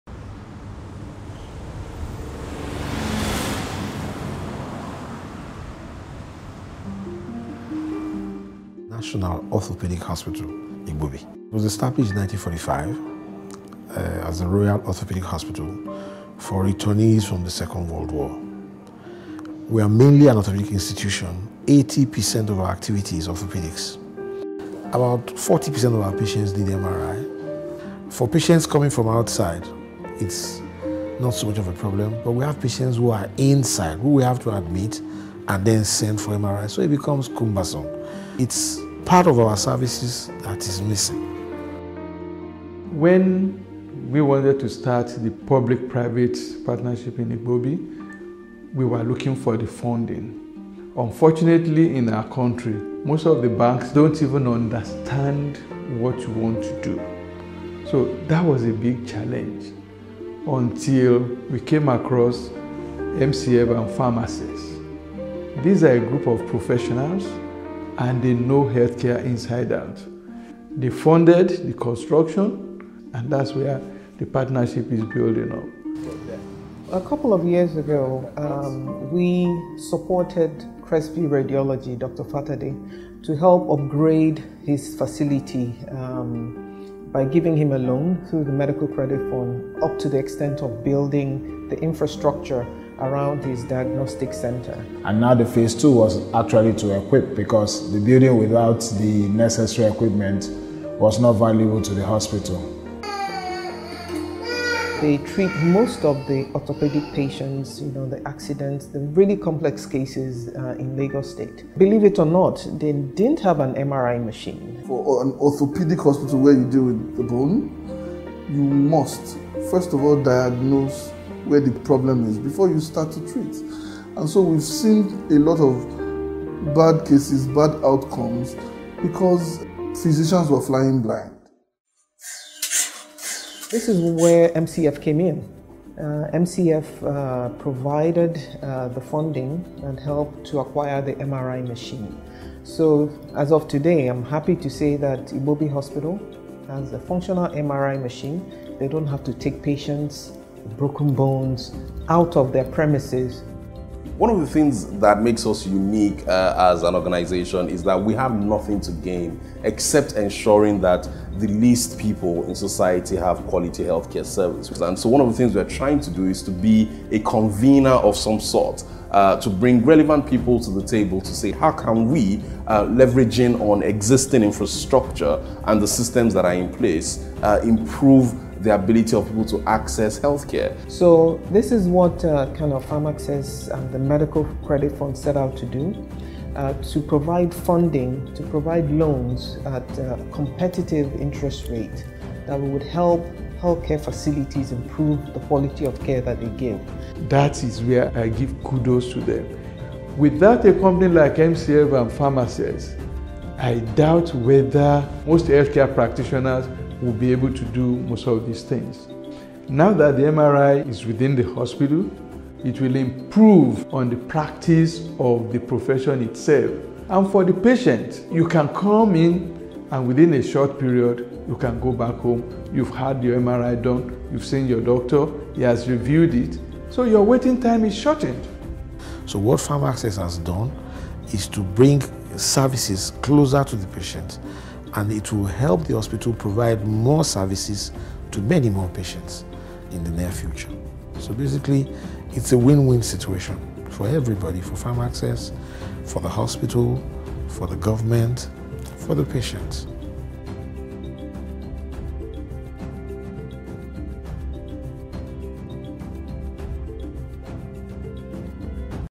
PharmAccess enables the purchase of medical equipment for the National Orthopedic Hospital Lagos
PharmAccess  Nigeria, Medical Credit Fund enables the purchase of MRI and CT Scan for the National Orthopedic Hospital Igbobi, Lagos